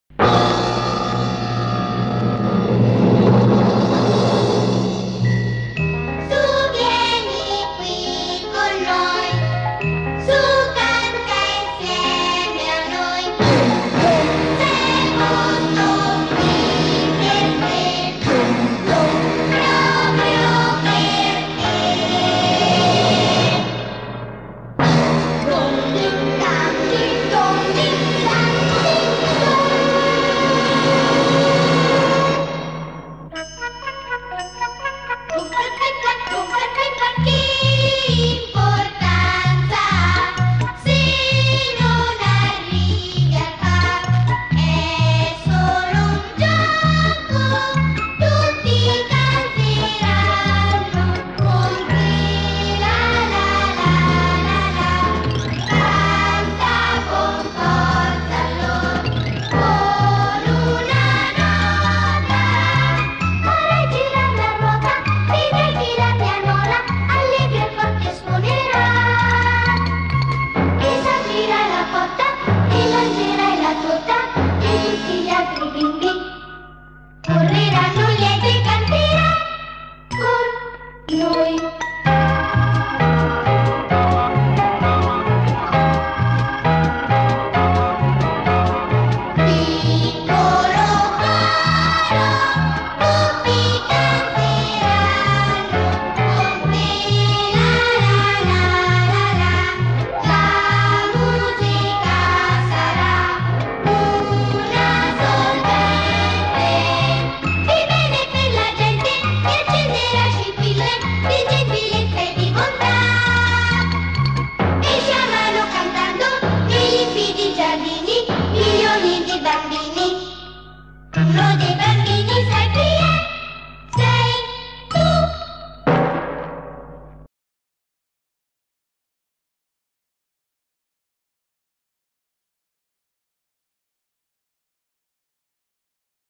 AUDIZIONI Piccolo Coro "Mariele Ventre" dell'Antoniano
Sono aperte le audizioni per entrare nel Piccolo Coro "Mariele Ventre" dell'Antoniano diretto da Sabrina Simoni.
Il 13 ottobre 2021 i bimbi di Bologna e zone limitrofe dai 4 ai 9 anni potranno cantare una canzone tratta dal repertorio dello Zecchino d'Oro o del Coro dell'Antoniano.
INFO: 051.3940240 dal lunedì al venerdì dalle 10 alle 12.